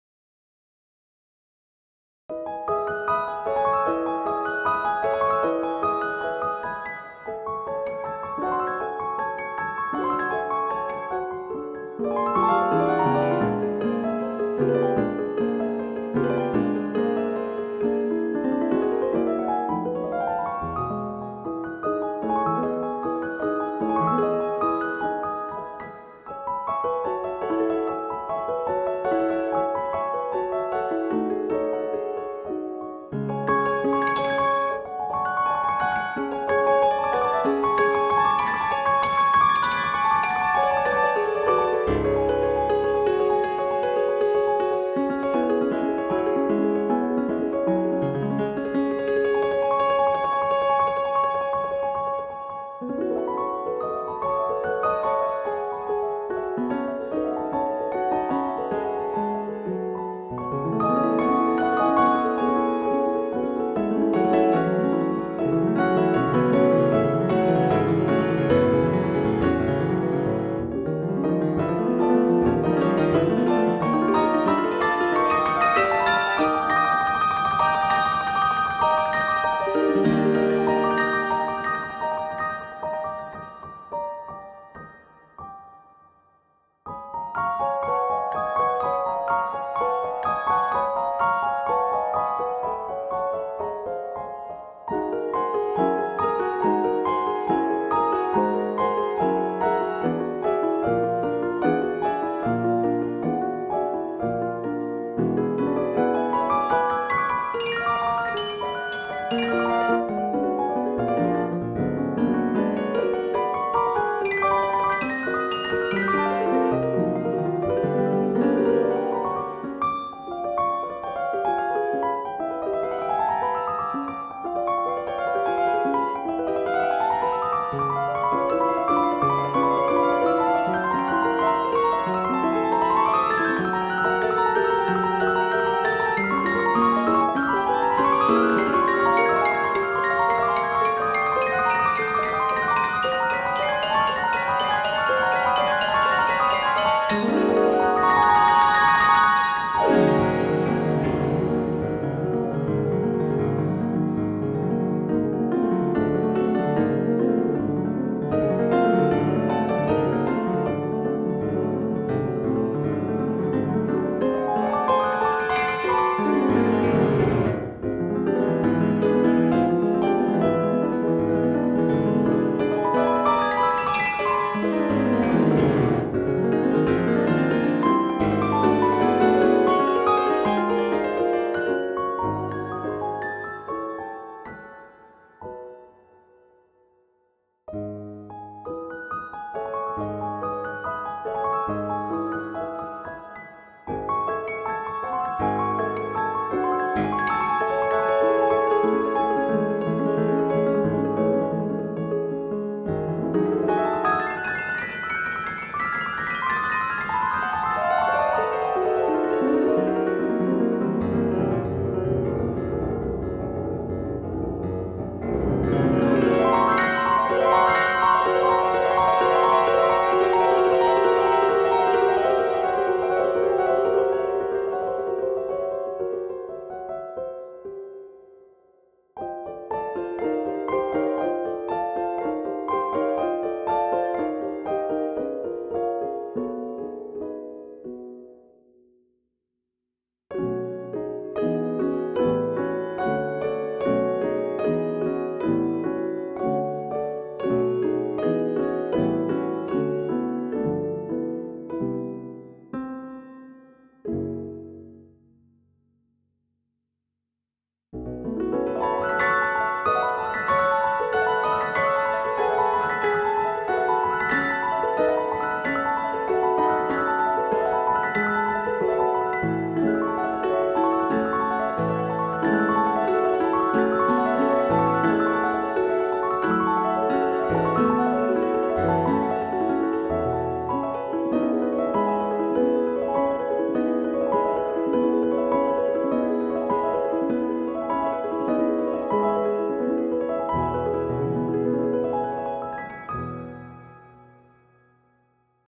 JAX Xperior Grand Piano - Expressive Impressionist
We selected Model 3 from the Xperior collection, an 'una corda' played piano with crystal clear timbre for an impressionistic piece, that sounds so magical, like from another universe with this.

Some parts have extraordinary high polyphony due to the pulsating and wide glissandi.

The JAX Xperior Grand features extraordinary timbres, that are not easily sorteable into any of the available categories. Such models may be used for special production needs and a sound, that is everything but common.